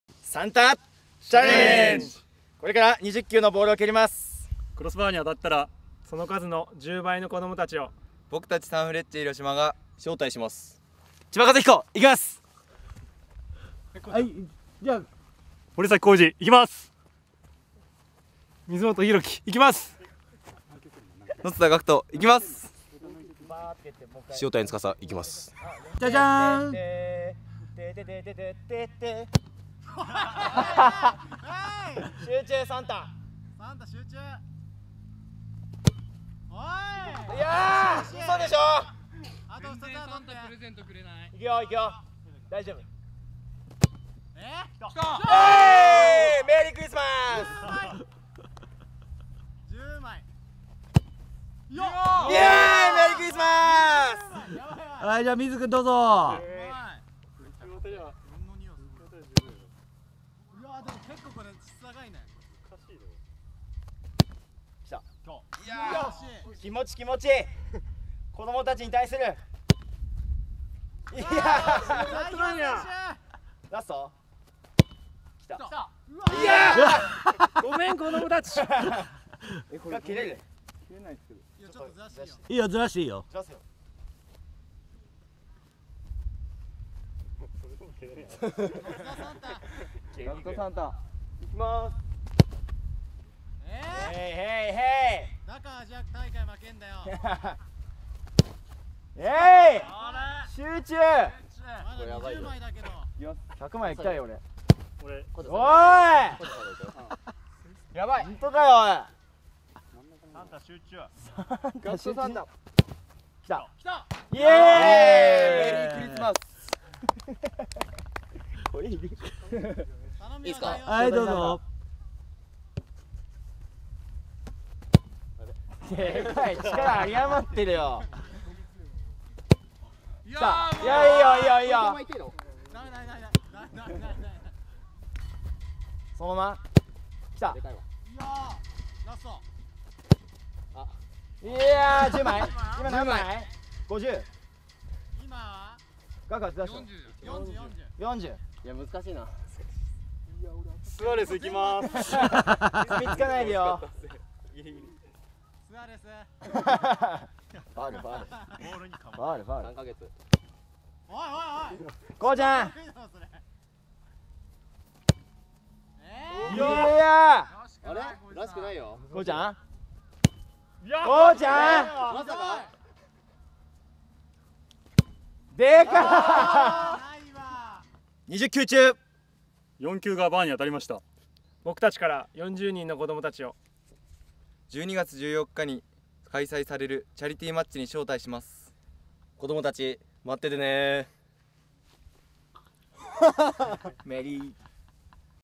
サンフレッチェ広島 サンタチャレンジ ！！ ①
サンタクロースチャレンジ！！

チャレンジャーが蹴ったボールが見事クロスバーに当たったら、その数×数倍のチケットを用意し、被災地の子どもをチャリティーマッチにご招待！！

さて、何本当てて、何人の子どもたちを招待できるのでしょうか！？